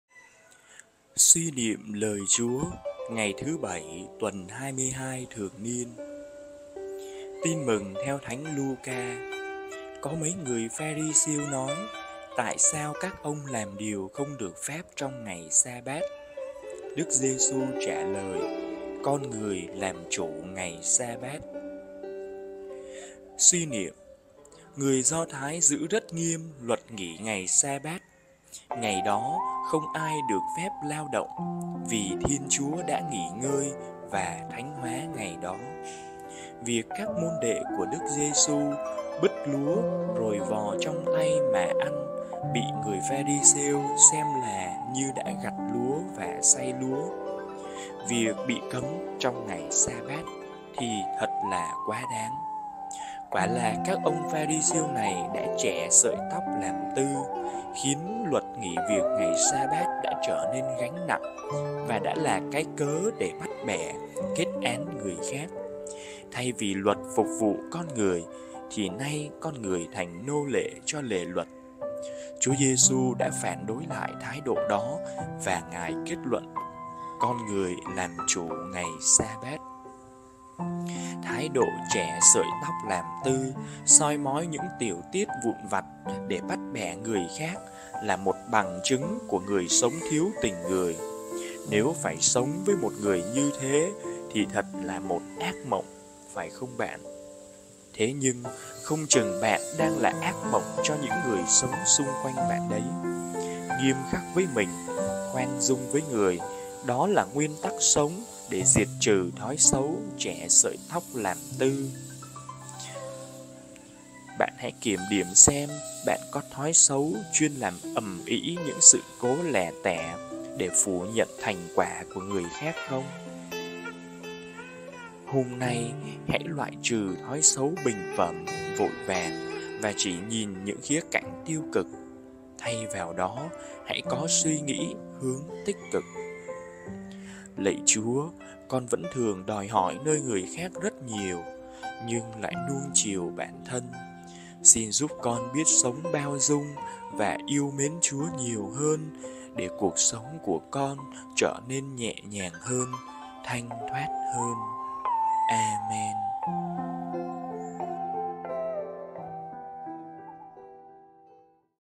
SUY NIỆM | Thứ Bảy 09/09/2023 | CHẺ SỢI TÓC LÀM TƯ?
Lm. Phêrô ĐINH QUANG MẠNH HÙNG, OP.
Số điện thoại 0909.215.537
♥♥♥ Xin chân thành cảm ơn Quý vị ♥♥♥